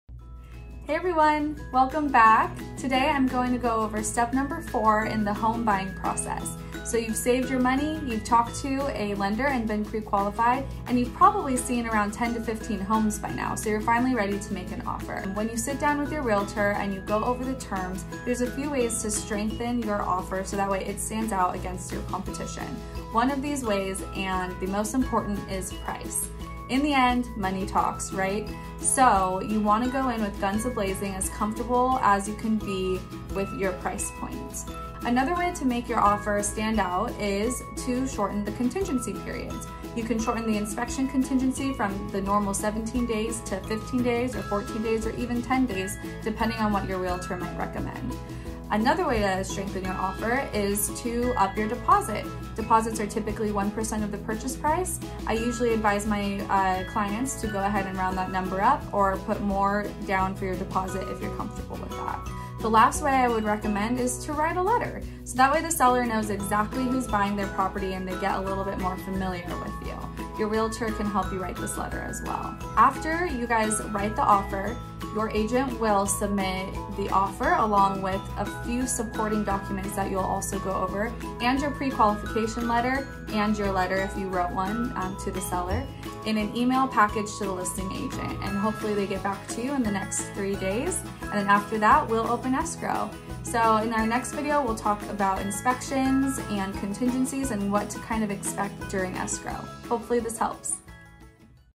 First Time Home Buyer - Step 4: Make an Offer!
How do you write a good offer that will get accepted? Find out what goes into the offer making process!